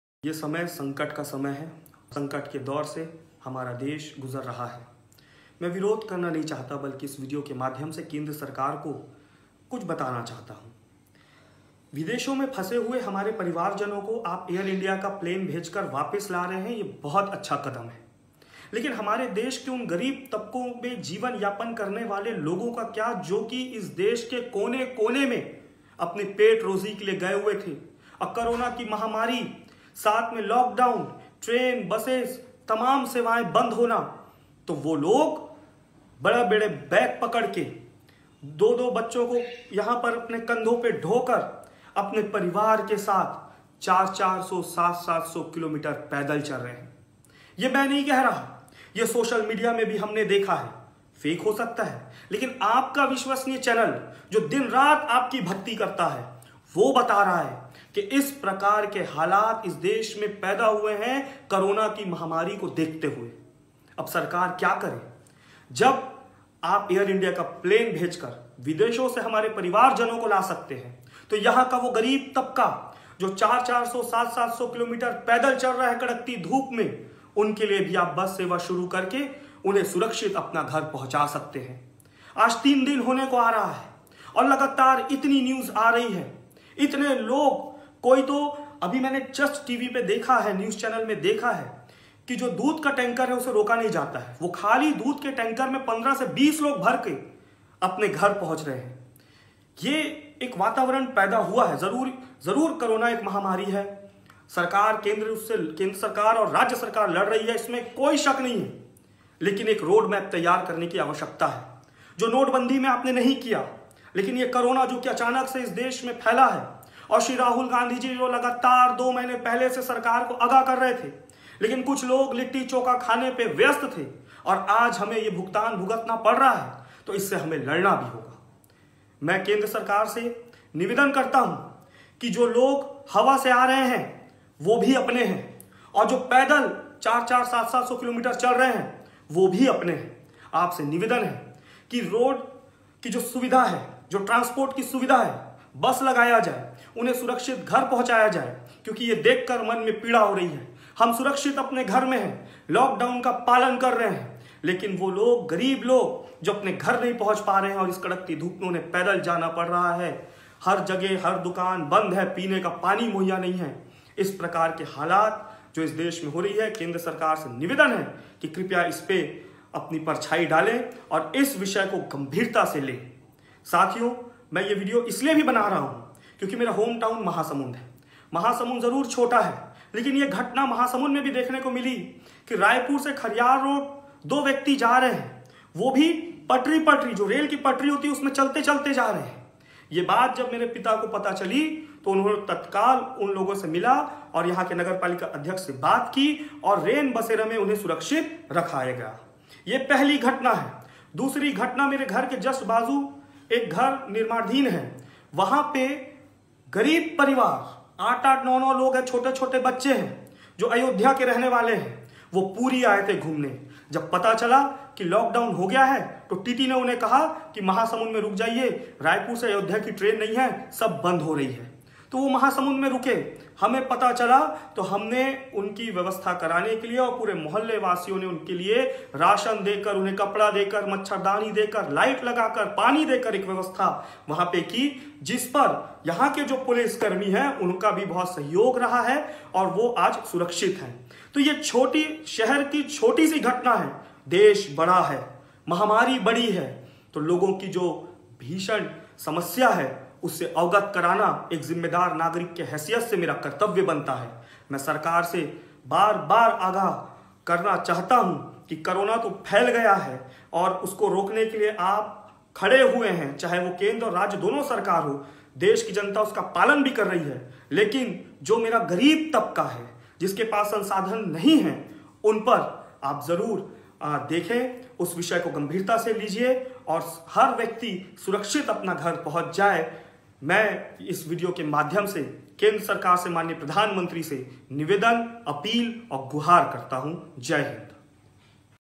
जो आसमान से लाए जाएंगे वो भी अपने हैं और जो जमीन पर मुश्किल में हैं ये भी अपने हैं..
विदेशों में फंसे भारतीयों को लाने के लिए अगर AirIndia का इस्तेमाल हो सकता है तो देश में पैदल चलकर अपने घर पहुँचने की कोशिश में लगे मजदूरों  के लिए भी तो सरकार कुछ कर सकती है,जो आसमान से लाए जाएंगे वो भी अपने हैं और जो जमीन पर मुश्किल में हैं ये भी अपने हैं..
नि: शुल्क ऑर्डर के लिए प्रोमो कोड arish अनवर कोई रिक्त स्थान सभी कैप का उपयोग करें